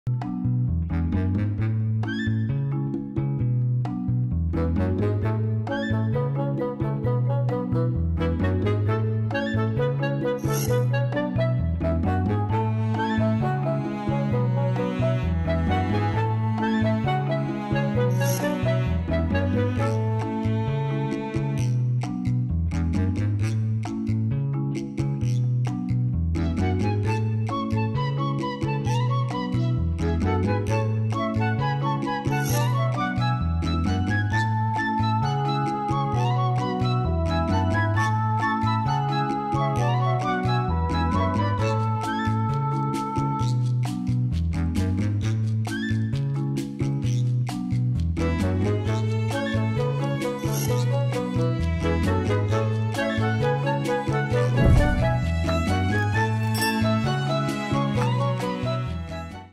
Bus Eater. SCP 2086 Bus with Clay
Hello everyone, it's Kamilla!

To sculpt the figures, I use regular and sculptural plasticine and aluminum wire for the frame. Here you can buy wire